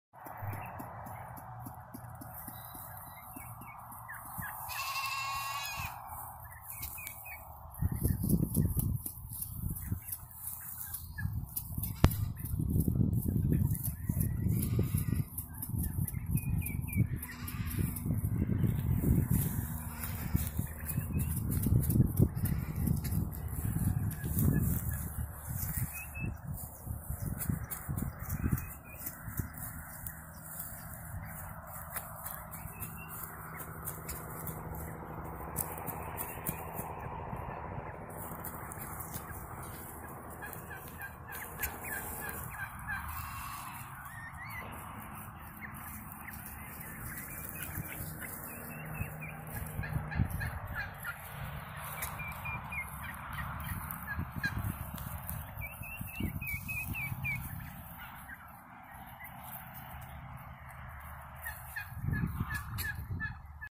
Slater Loves to Dance for the Ladies
I caught Slater doing the mating dance. It always makes me giggle.